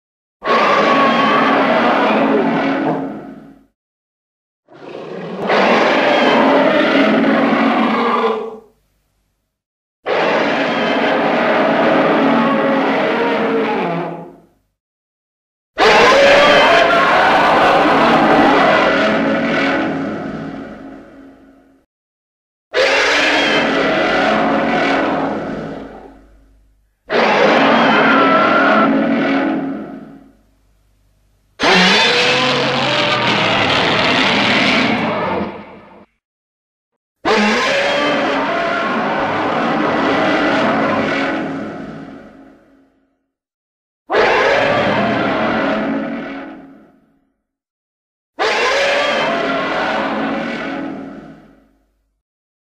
Camarea The Giant Chameleon (Bride of Godzilla) Sound Effects
This is another kaiju that was scrapped in the Bride of Godzilla